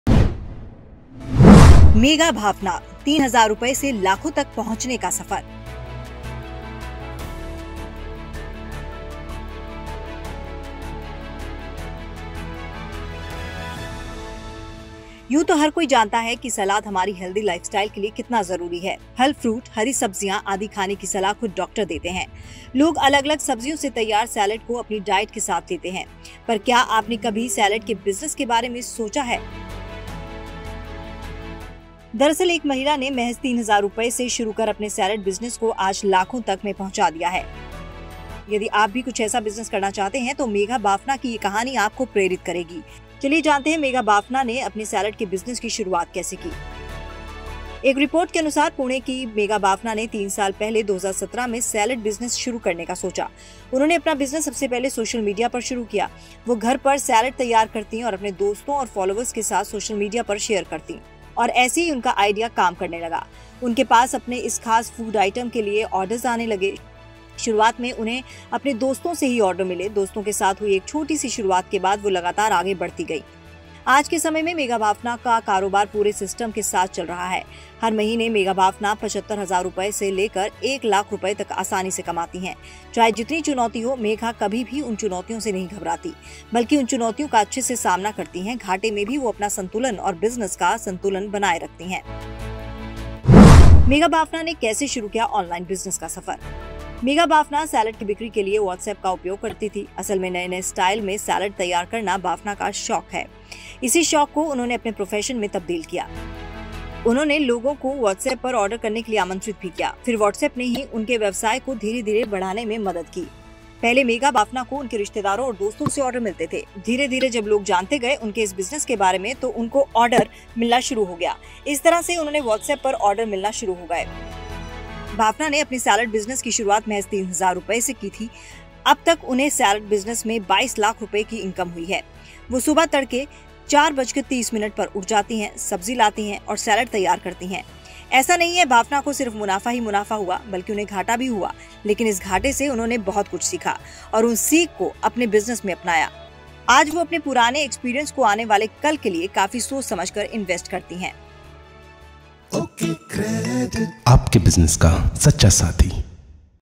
Megha Bafna 3,000 रुपये से लाखों तक पहुँचने का सफ़र | Pune Woman Earns ₹1.5 Lakhs Per Month
यदि आप भी कोई ऐसा बिज़नेस आइडिया ढूँढ रहे हैं, जिसमें आप अपनी पसंद को शामिल कर सकें और अपनी पसंद से पैसा बना सकें। तो आपको इस वीडियो को पूरा देखना चाहिए। इस वीडियो में आप देख सकते हैं की अपनी पसंद को आप कैसे अपने प्रोफ़ेशन में बदल सकते हैं।
यह कहानी एक ऐसी महिला की है जिसने अपनी पसंद को अपने प्रोफेशन में बदला, साथ ही आज कामयाबी की सीढ़ी चढ़ रही हैं। जी हाँ हम बात कर रहे हैं, 'मेघा बाफना' की, जिन्होंने महज़ 3 हज़ार रुपये से अपने बिज़नेस की शुरूआत की।